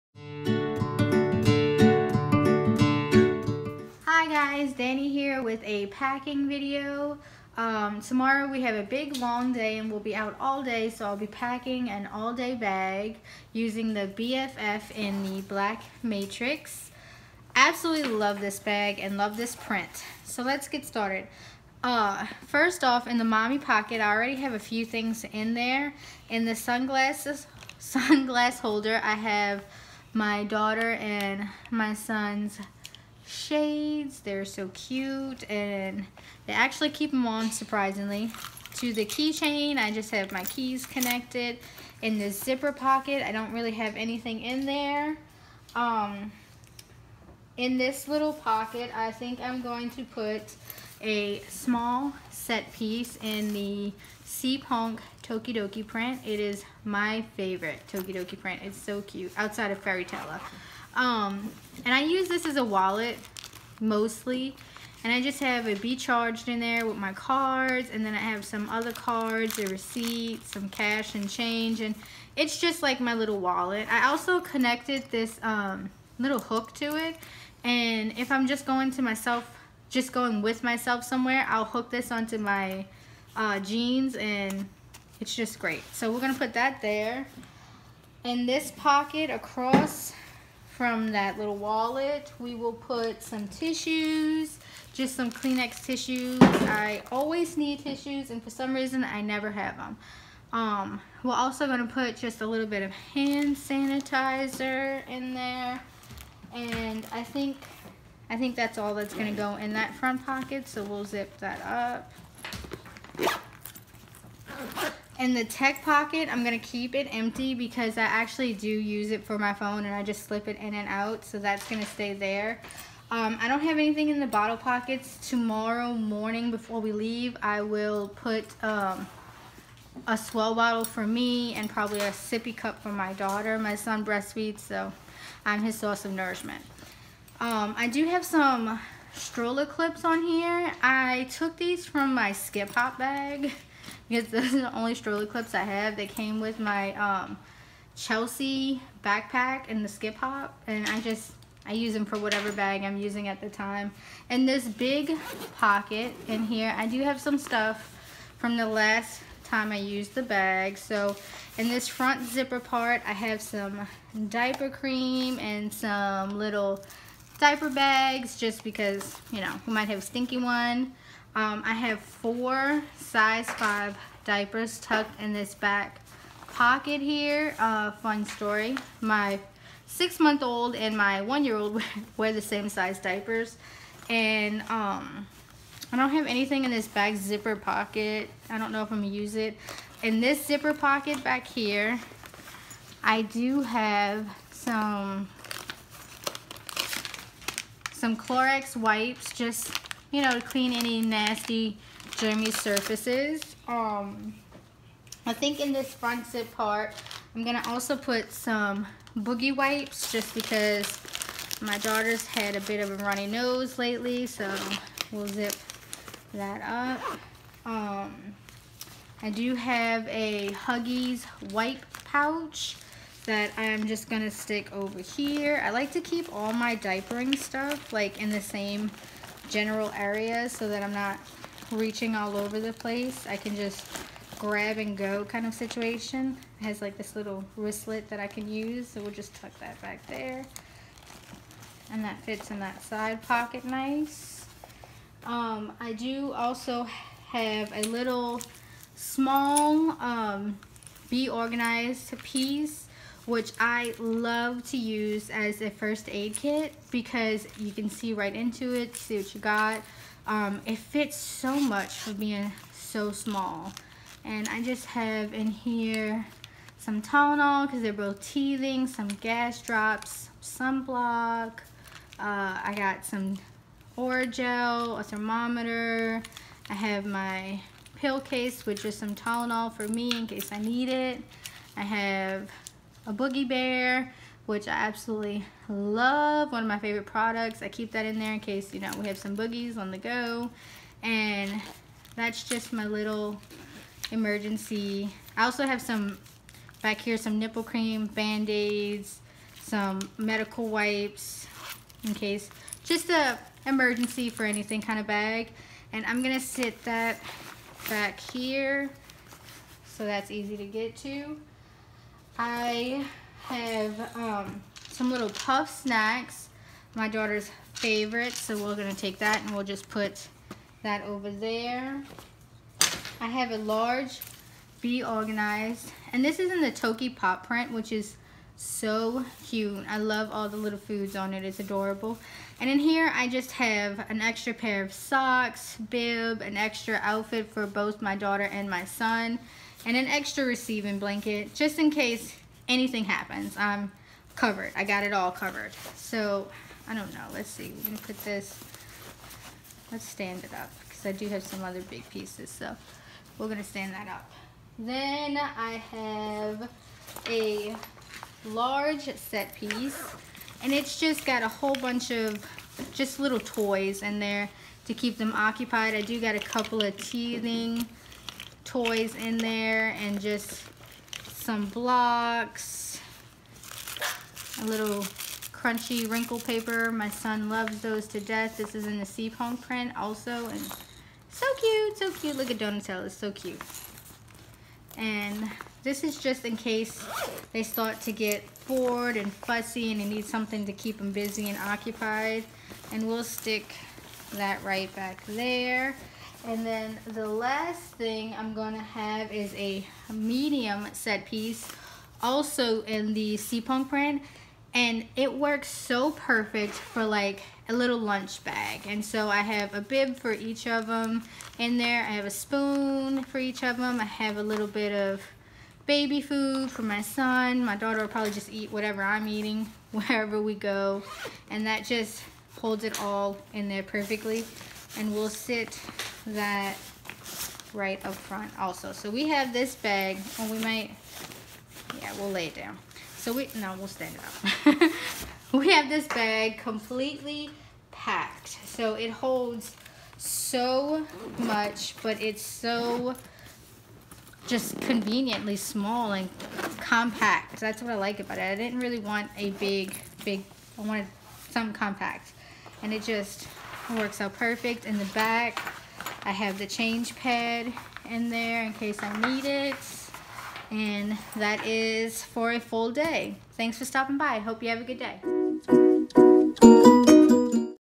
Packing Video Jujube BFF
Packing Video of the Jujube BFF! Hey, guys this a pack for an all day trip! Hope you like it!